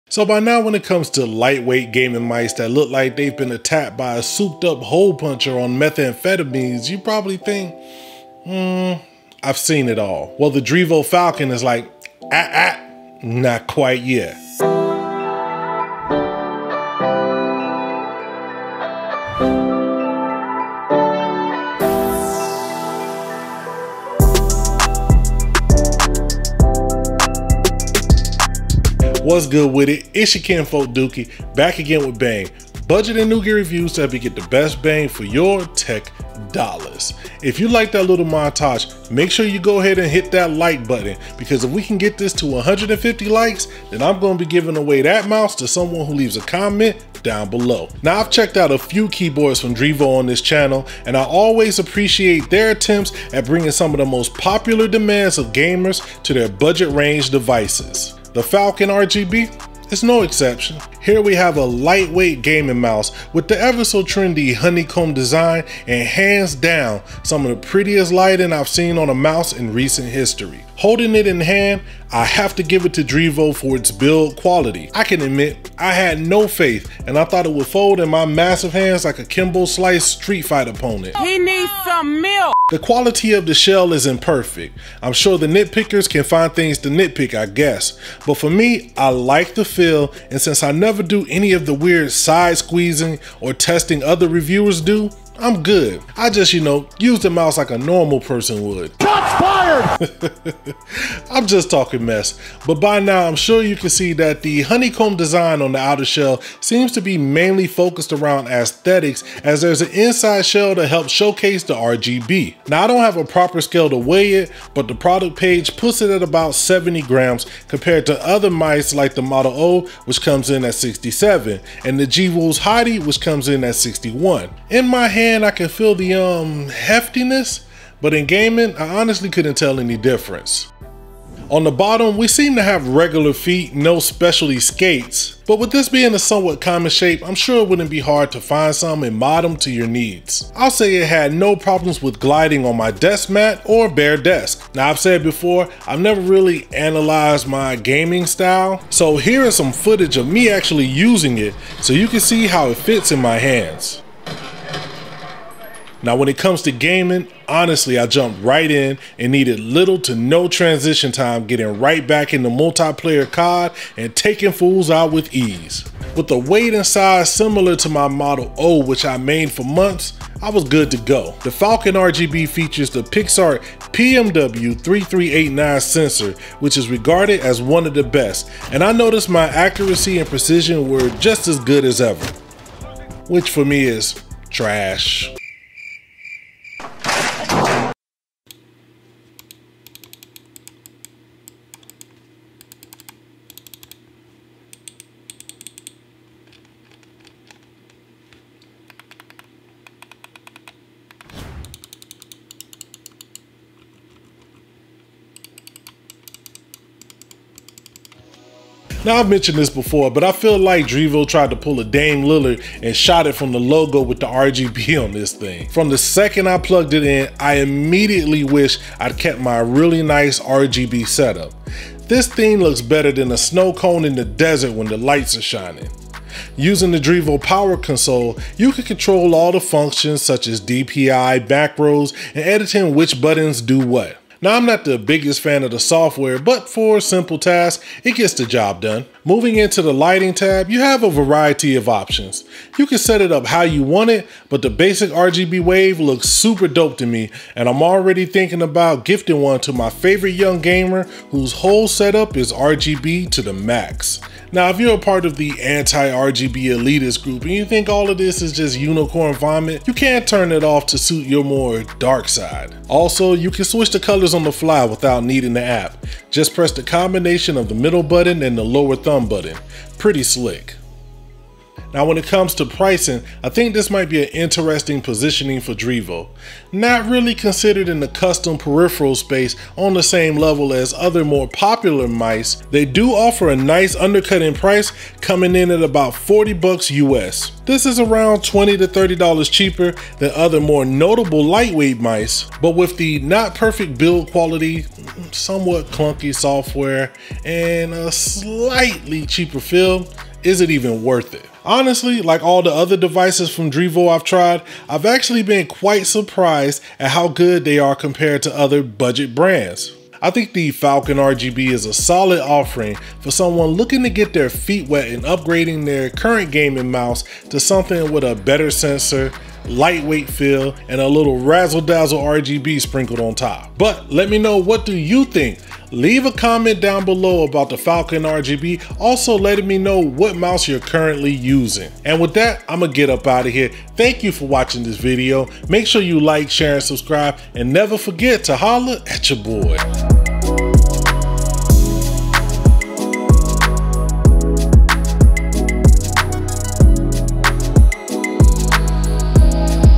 Most BEAUTIFUL RGB Mouse | Drevo Falcon
The Drevo Falcon RGB is a take on the lightweight honeycomb gaming mice from a "budget" perspective, let's see what's good.

Drevo Falcon RGB

***Looking for the best Tech Deals and Trusted Opinions, try the Lustre browser extension for FREE

Thumbnail  artwork done by Saad Shah. Check out and contact at Gargoyles At Work on Youtube.